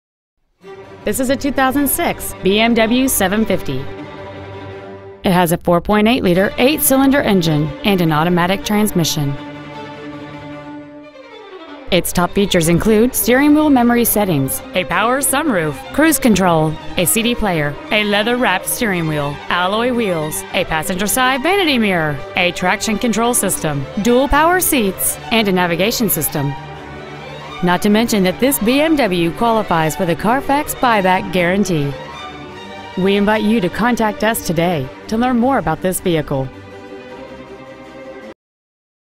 2006 BMW 750 Monrovia CA 91016
Year: 2006
 Make: BMW
 Model: 750
 Engine: 4.8L V-8 cyl
 Trans.: Automatic
 Exterior: Black
 Miles: 84,261
 Interior: Black

 1425 S. Mountain Ave.

 2006 BMW 750 Monrovia CA